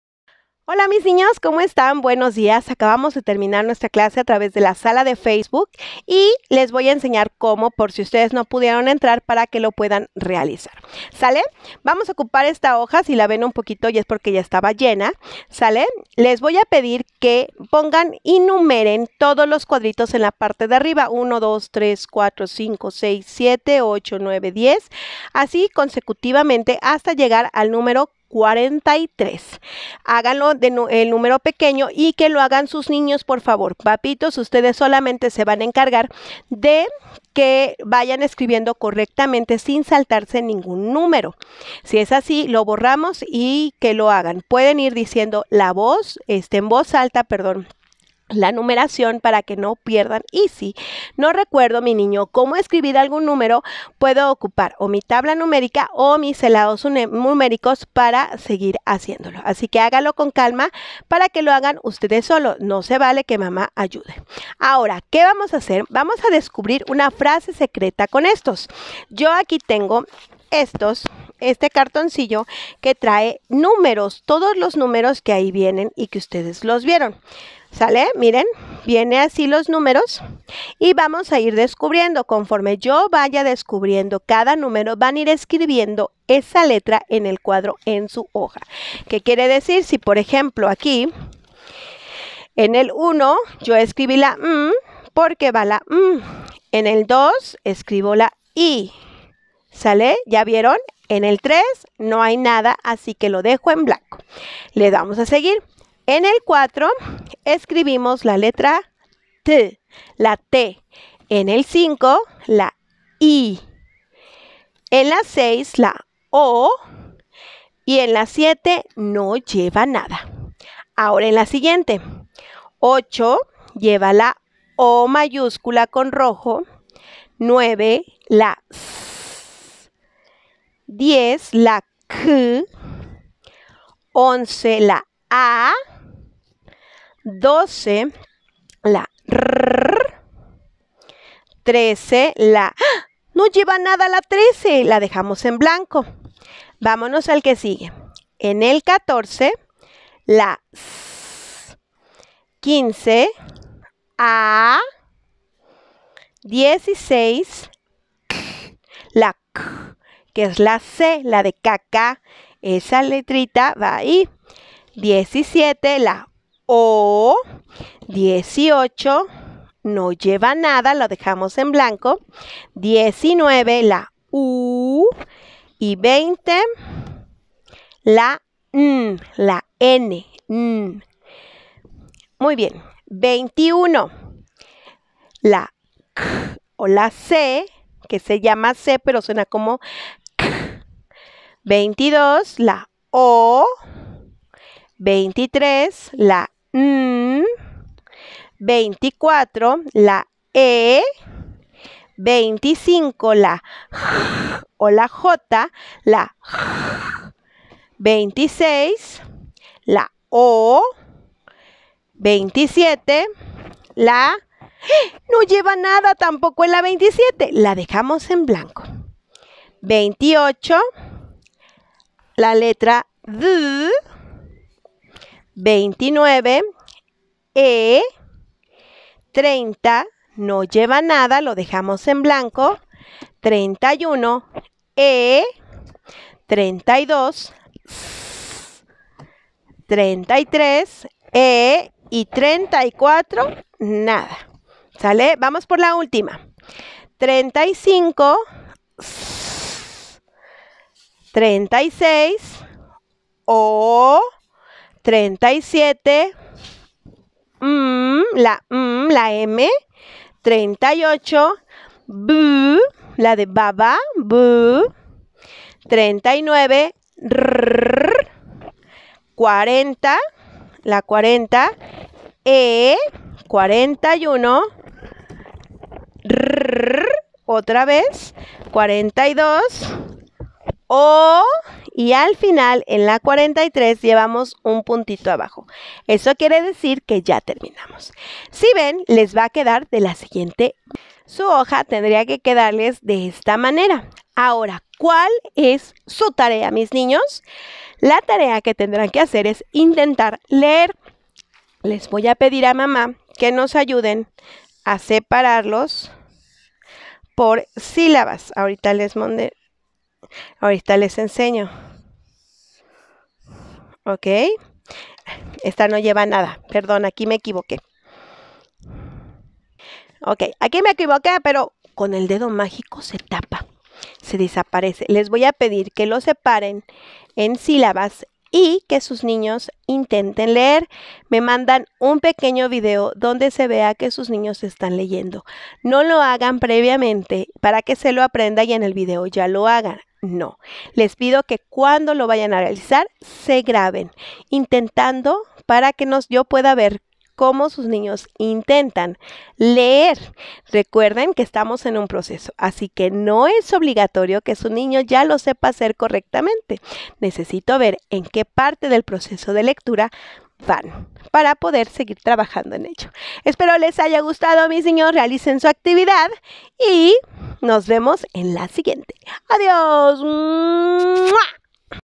Actividad 177. Detectives de sílabas. Serpientes y escaleras.
Preescolar. Lenguaje y Comunicación. Pensamiento matemático. Cálculo Mental.
Actividad 177. Detectives de sílabas. Serpientes y escaleras. Actividad realizada por Sala de Grupo de FB del salón.
Maestra Eve